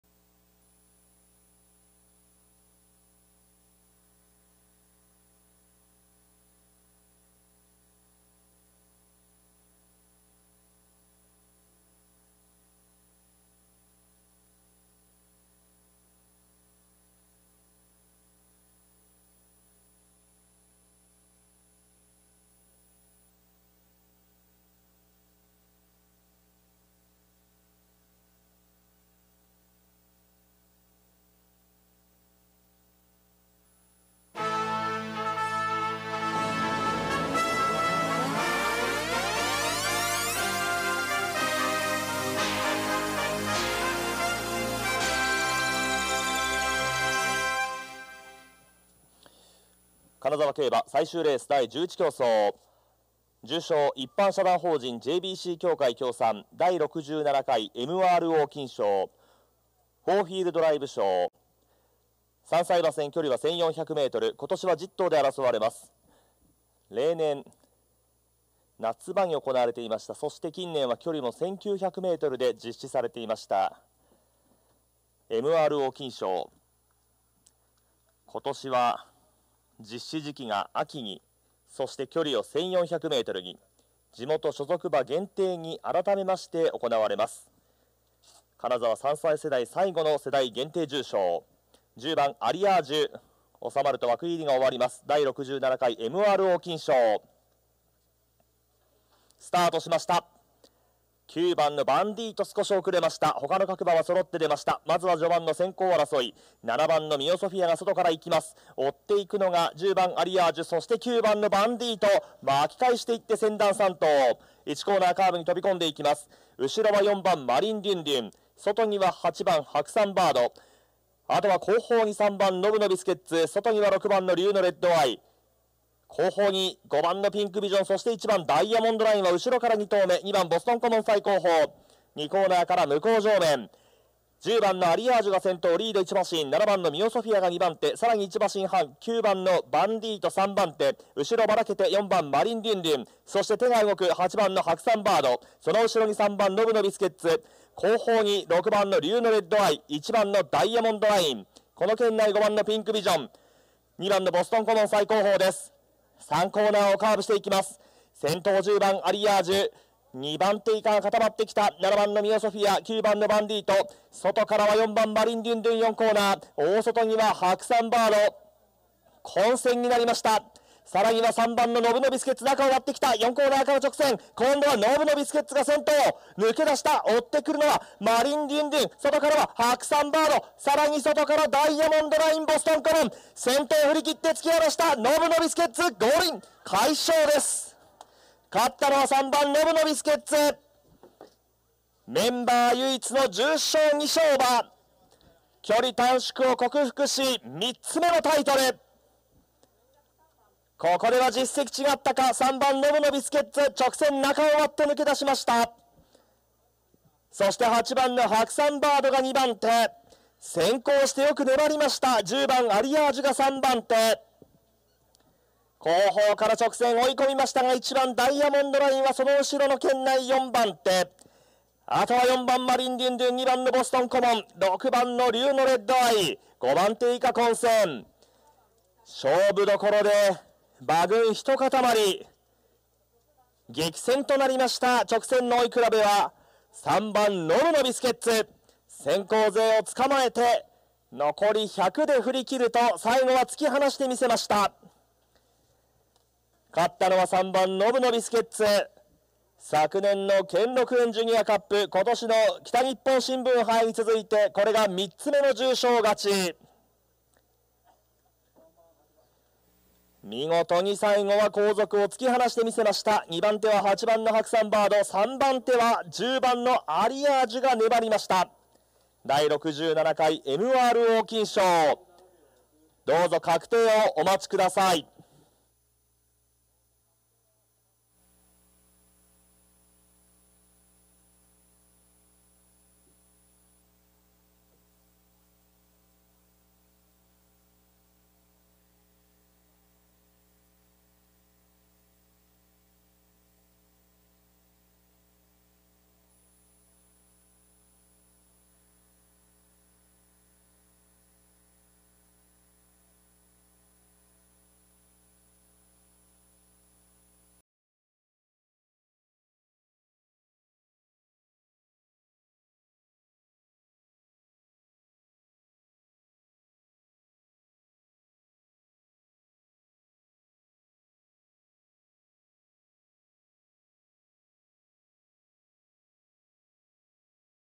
2023.10.15. 金沢11R ［重賞］ 第６７回 MRO金賞(フォーウィールドライブ賞)
金沢12回1日 11R（一社）ＪＢＣ協会協賛　[重賞] 第６７回 ＭＲＯ金賞（フォーウィールドライブ賞）サラ系３歳(定量)(混合) ダート1,400m 右 天候：雨　馬場：重

１着③ノブノビスケッツ(金沢)青柳正義 1:27.6